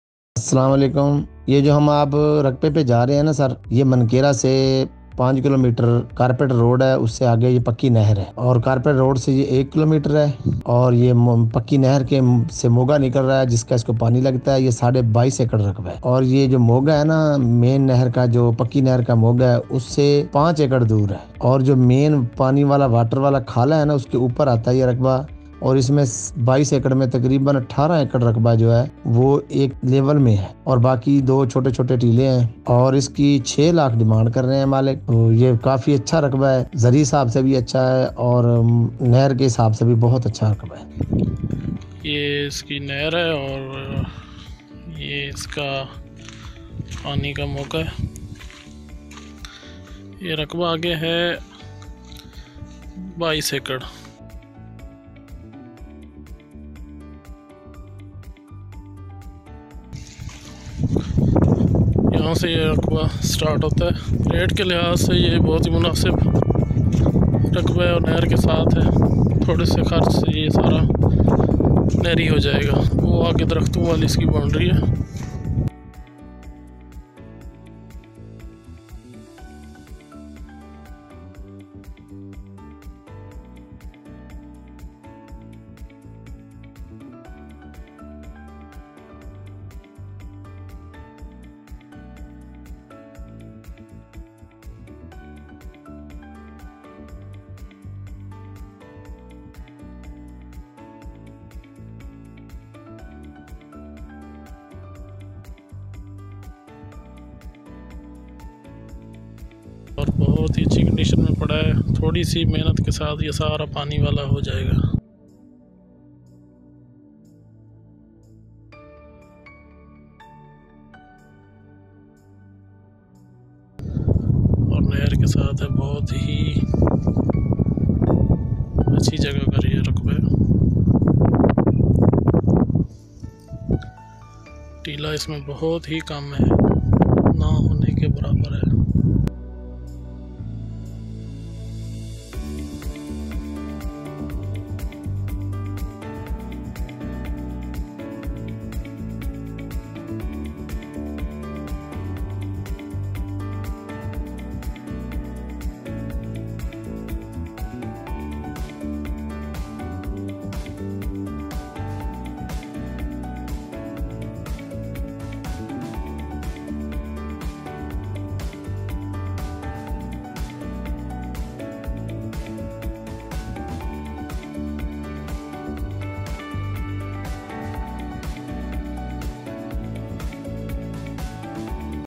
Cheap Agriculture land for sale in Pakistan | Low price land | Bhakkar Property
Agriculture land for sale in Pakistan Cheap land Low price land  Bhakkar Property
Todya new video 22 acer land in Mankera Bhakkar for sale demand 600000